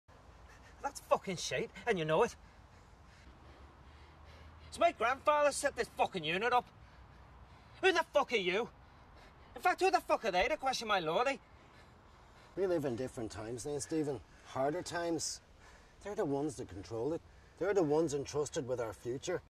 BULLSHIT
Howard Corlett as Steven McPherson - a paramilitary accused of collaboration with the authorities.